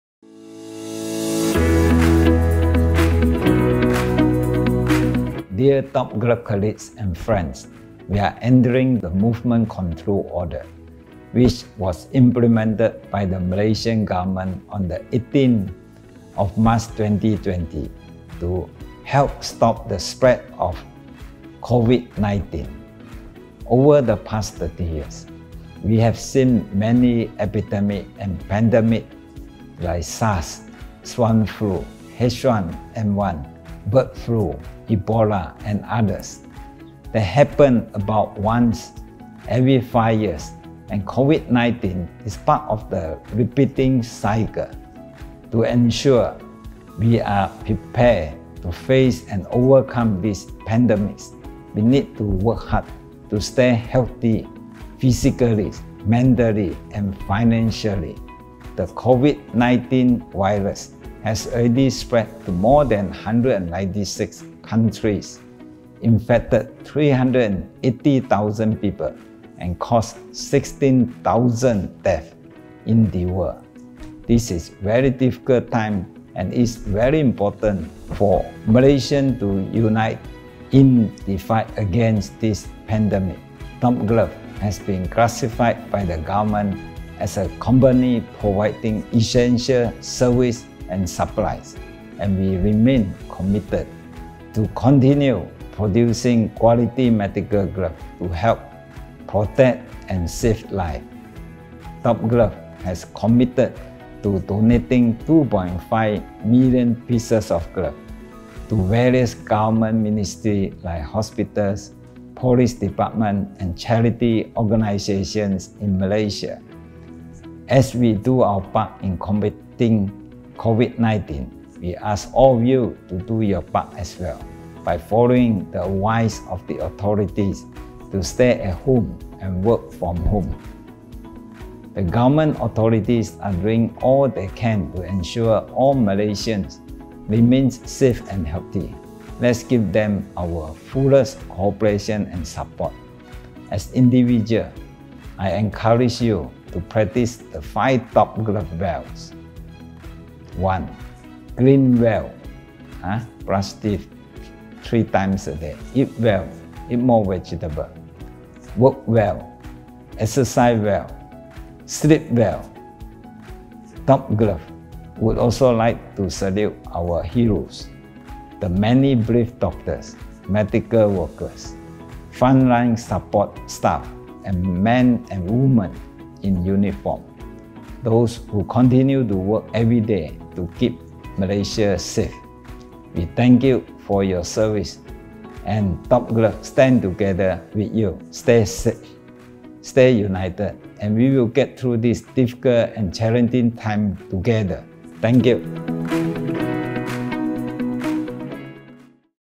Top Glove: Tan Sri Dr Lim Wee Chai's Special Message on COVID-19
A special message by Tan Sri Dr Lim Wee Chai on COVID-19 recorded on the 25th of March 2020